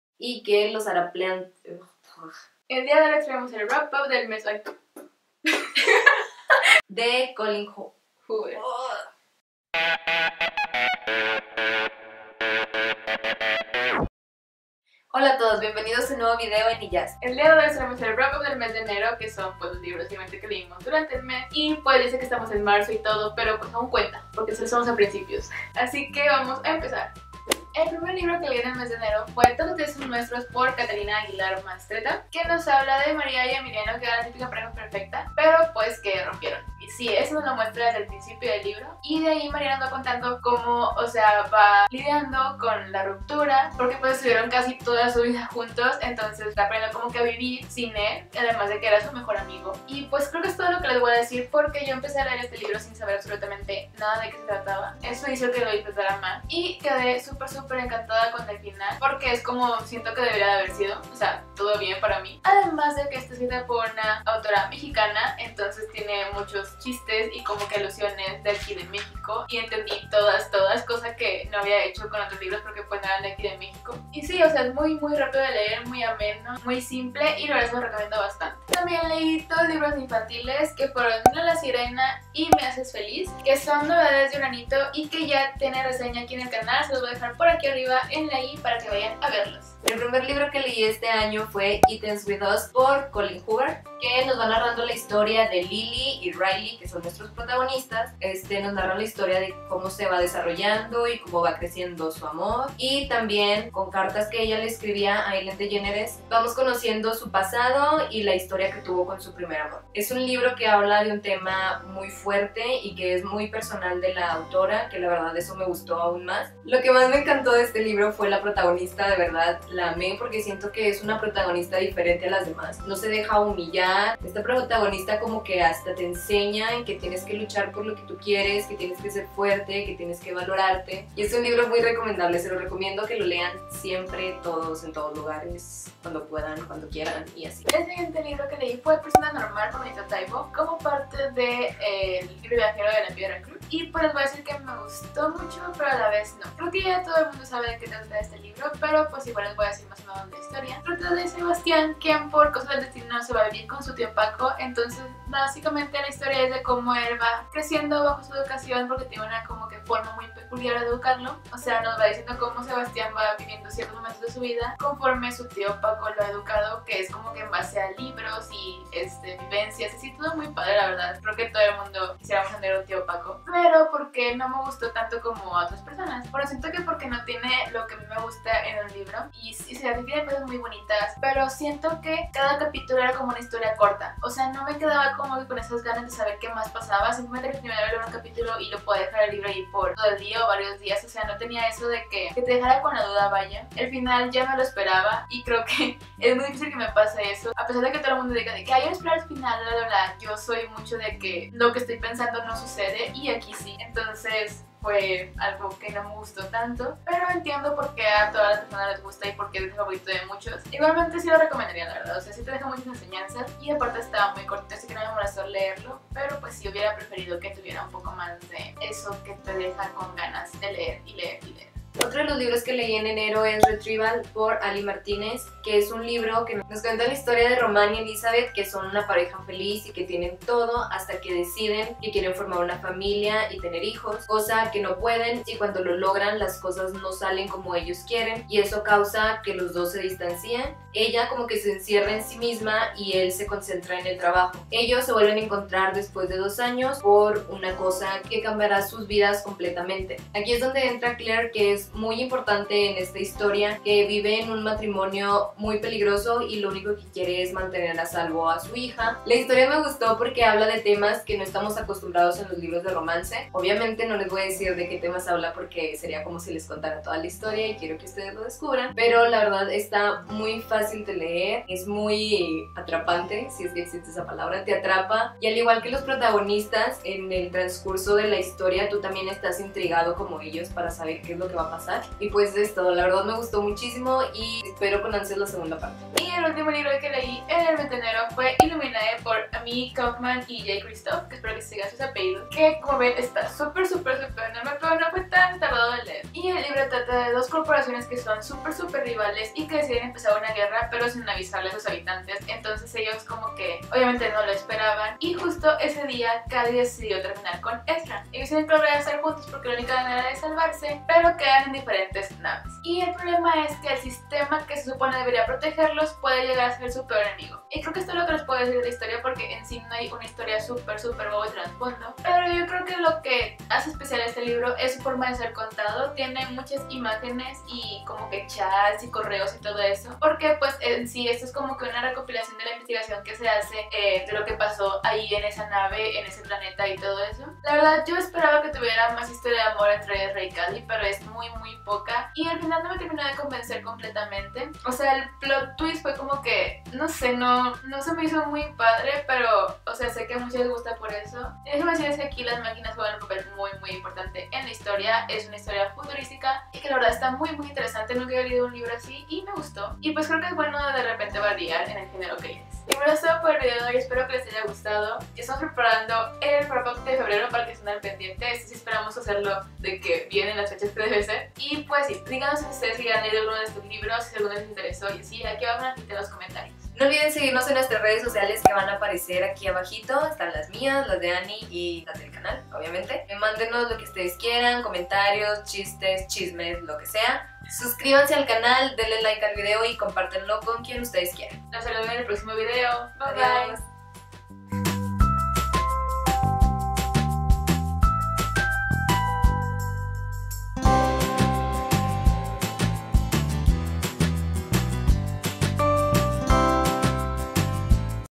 WRAP UP ♥ ENERO (Illuminae, Persona Normal, IEWU, Retrieval, TLDSN) || IJASV
¡¡HOLAAAAA!! Después de mucho tiempo, por fin subo el video a tiempo... estooy demasiado feliz xD peeero bueno, como ven en el video les traemos un resumen de los libros que leímos en el mes de Enero :B espero que les guste mucho y recuerde que iremos subiendo las reseñas más completas en nuestro blog c:
¿Ya han leído alguno de estos libros? ¿Qué les pareció?